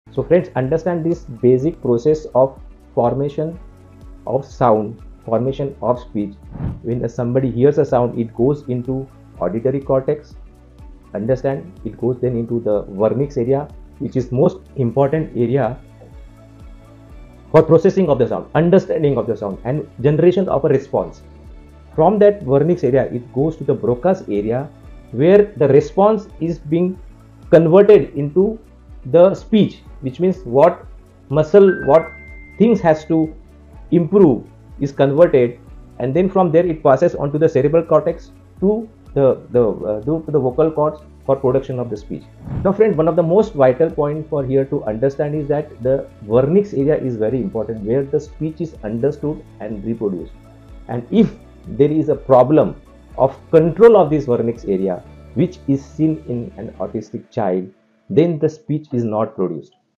Understanding the Brain's Role in Autism Speech
Understanding the brain's role in autism speech challenges can unlock key insights into speech development. Dr. Pravin Jain explains how Wernicke’s and Broca’s areas process speech comprehension and production in neurotypical children. However, in autistic children, disruptions in these areas can lead to echolalia or delayed responses.